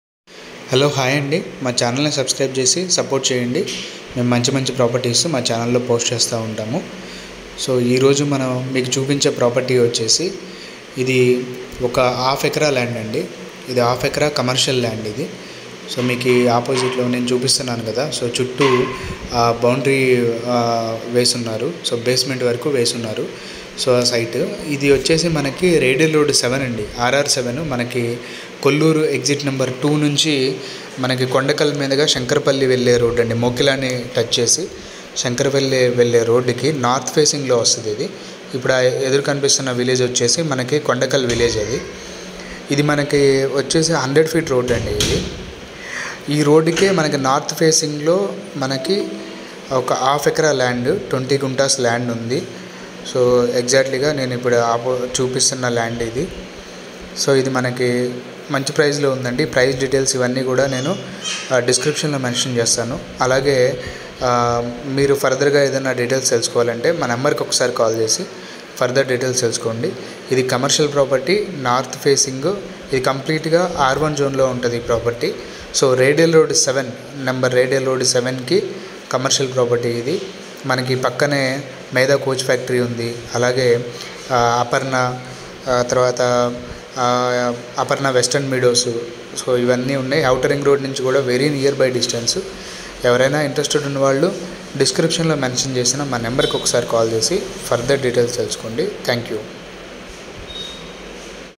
20Guntas RR7 100Feet Road Facing land for sale
20guntas commercial Land for sale

location:RR7 kondakal
facing: North
price: 55lakhs per gunta